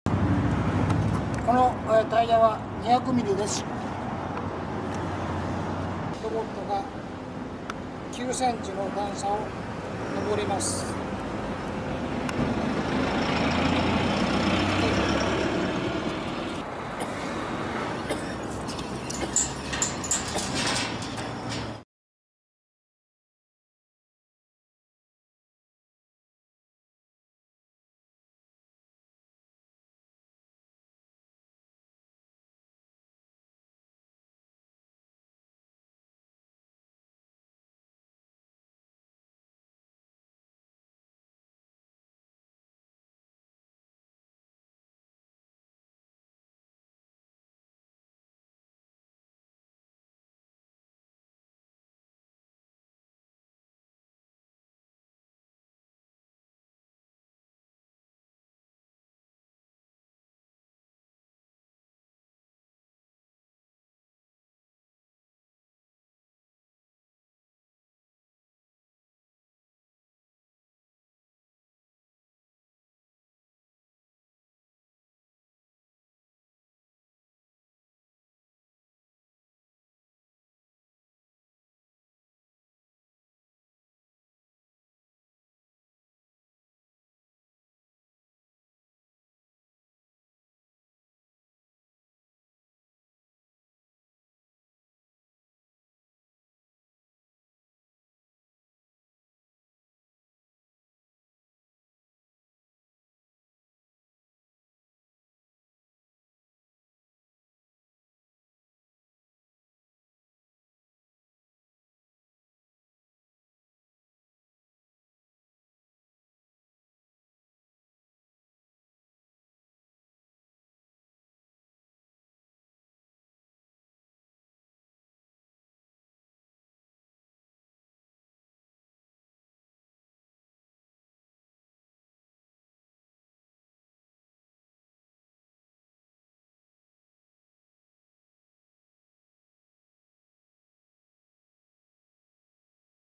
【日本タップ】平面展開 ロボカム-2800(Φ500～900mm管径対応タイプ) 9cmの段差を登ります。
ロボカム‐２８００は、φ150～2800mm管に対応します。7/30～8/2開催の下水道展'13東京に出展しますので、ご覧ください。標準価格は、￥１４，２５０，０００です。詳細は、日本タップ・ホームページ【